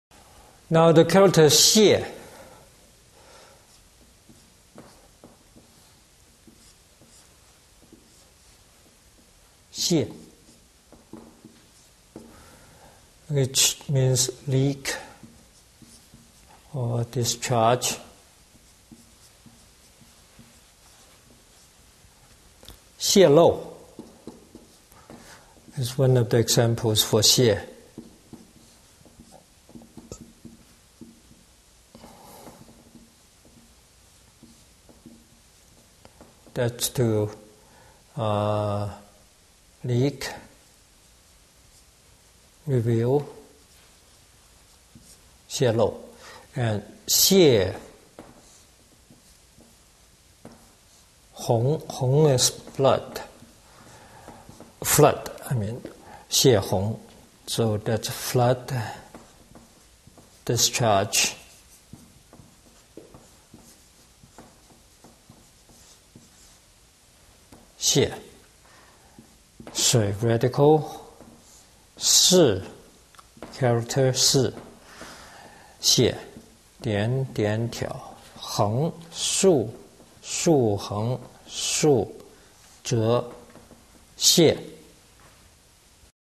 Chinese Symbols for Leak
Chinese Symbols for Leak. Part of the series: Chinese Radical Shui. Learn the Chinese symbols for &quot;leak&quot; with tips on writing and pronouncing Chinese characters from a Chinese language specialist in this free video on Chinese words and phrases.